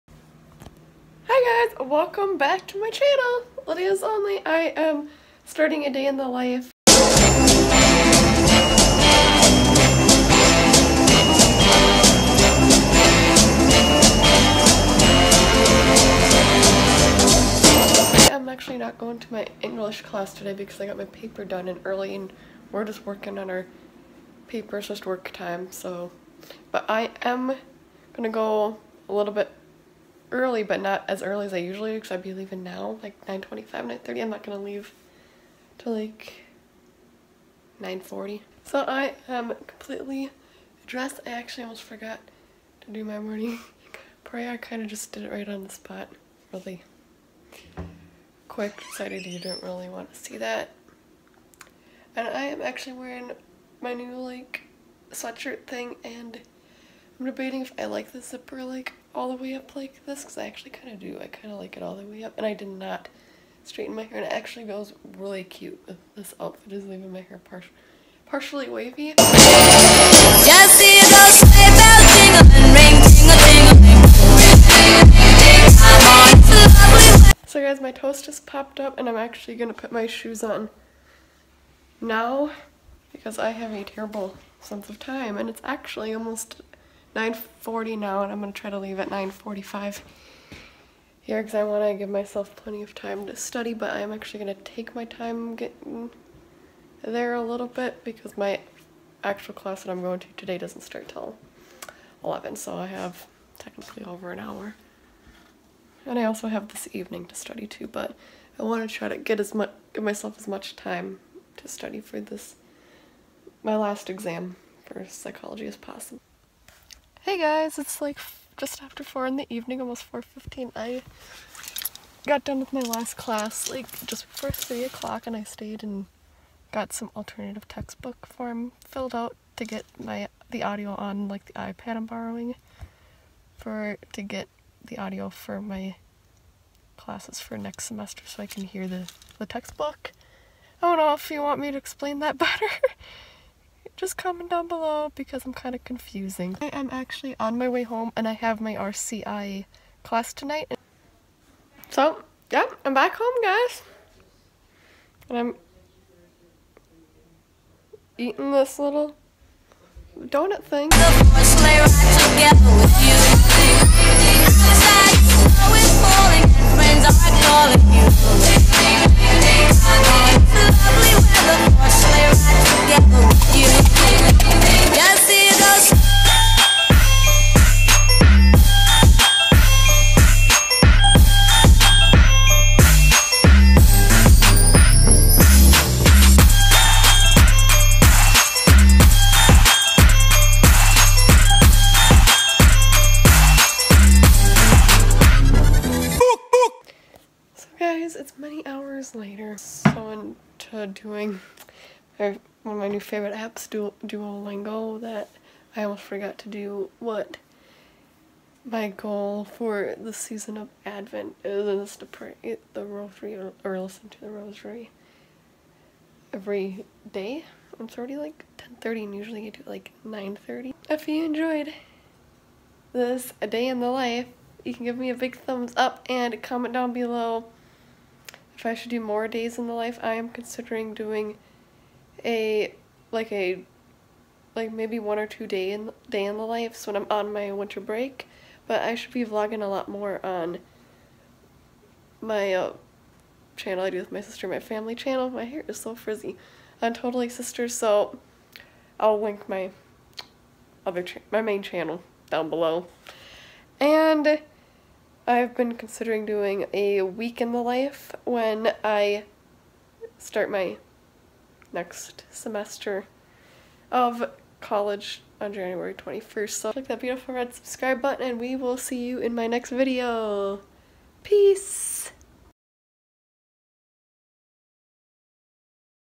DAY IN THE LIFE | LAST TUESDAY BEFORE WINTER BREAK! | LydiasOnly
I am back! I have been so busy with college, work, church, and just life. I really enjoyed filming this day in the life. My winter break from college is Dec 17 to Jan 21, 2019 :)